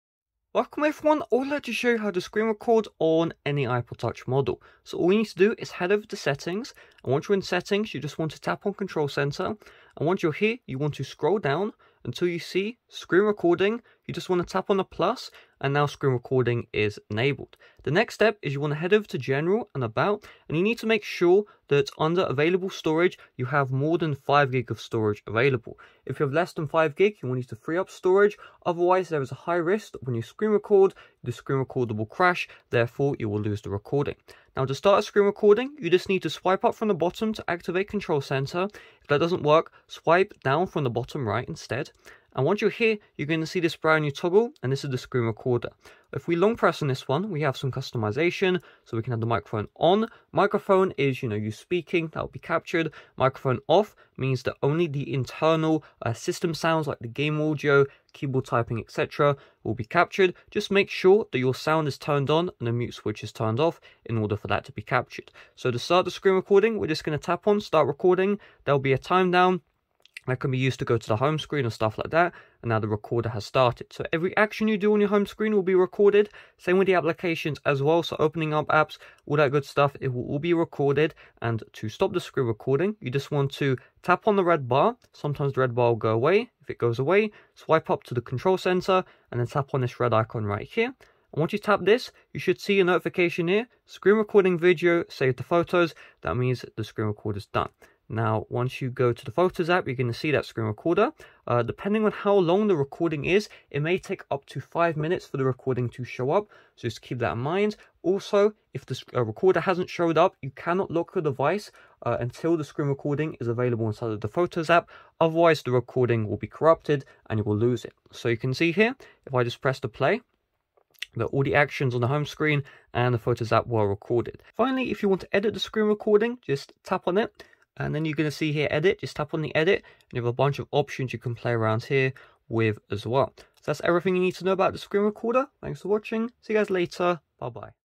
How To Screen Record on any iPod Touch model | Full Tutorial
How To Screen Record on iPod Touch | Easily record your iPod Touch screen! Do you own any model of the iPod Touch and want to screen record the screen of the iPod? If so, this video is for you! I will teach you How To Screen Record on iPod Touch in this tutorial. No matter what iPod Touch model you own, this guide will show you how to setup the iPod Touch Screen Recorder, and how to have your screen recorded! This means that if you own the iPod Touch 6, iPod Touch 7 or future iPods like the iPod 8th Generation or even 9th Generation, the process to Screen Record is the exact same across all iPod Touch models!